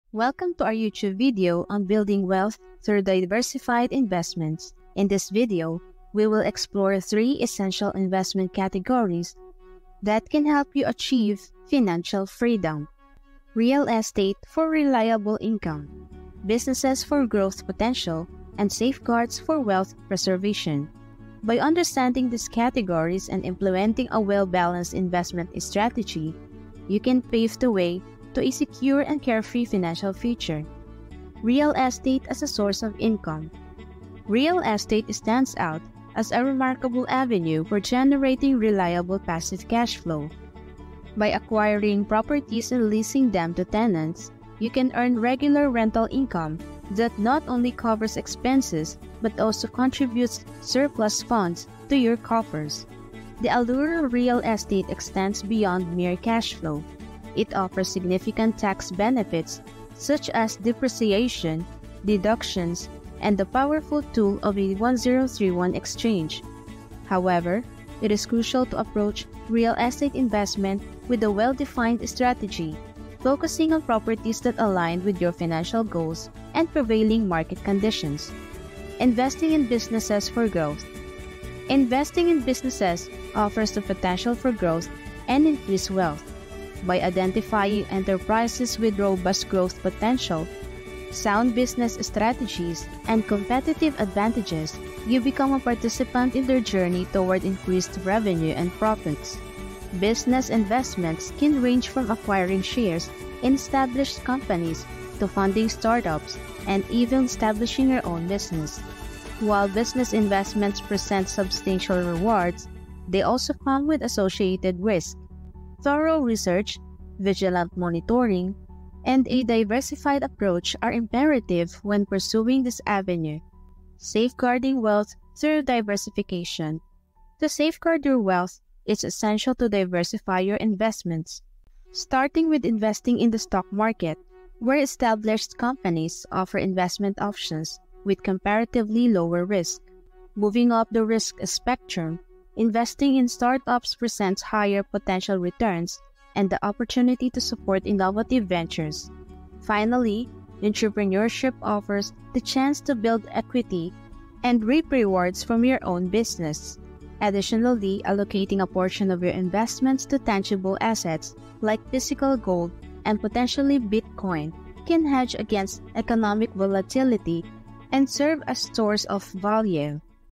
The Ultimate Wealth Building Guide: Unlocking Investment Secrets
In today's video, we unveil 'The Ultimate Wealth Building Guide,' providing you with the key to unlocking investment secrets that can change your life.
🔍 Are you wondering how to make your money work for you instead of worrying about it? You're not alone. Many people seek financial security and long-term prosperity.
💼 One of the strategies we'll explore is the power of diversification. Discover how to safeguard your wealth through smart investment choices. Learn how spreading your investments across various asset classes, such as real estate and businesses, can minimize risks and maximize your returns.
🏡 Real estate is often a lucrative source of income. We'll delve into the benefits of real estate investment and how it can provide a consistent revenue stream while potentially appreciating in value over time.
🚀 Additionally, we'll share three key investment avenues you should consider to accelerate your wealth-building journey. These investments have the potential to generate substantial returns and boost your financial well-being.
🧠 In our quest to help you master wealth building, we'll also provide a crash course on 'How Not to Worry About Money Again.' The right knowledge and strategies can free you from financial stress and lead you towards financial peace.
🔒 So, if you're eager to embark on a transformative financial journey, join us as we unlock the secrets to building wealth and securing your financial future.